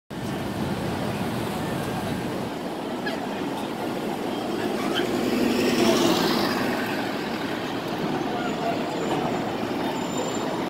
2021 New Flyer XDE40 #9580 on the M7 at Times Square
Manufacturer: New Flyer Industries
Model: XDE40
Year Model: 2021
Type: Hybrid
Length: 40ft
Width: 102 inches
Engine: Cummins (EPA-21) ISB 6.7
Transmission: BAE Systems HDS300 Series-E Hybridrive
Depot: Mother Clara Hale
Number: 9580
Route: M1
Status: Brand New